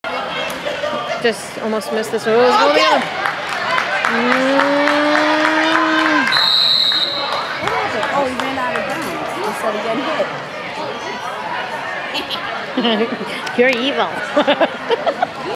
Michael Carter AWOL on the backside
Winthrop Vikings Winter Football C Team vs. Nor-Rock Blue, at NH Sportsplex, Bedford, NH, Feb. 15, 2014 (Winthrop Vikings 38, Nor-Rock Blue Vikings 14)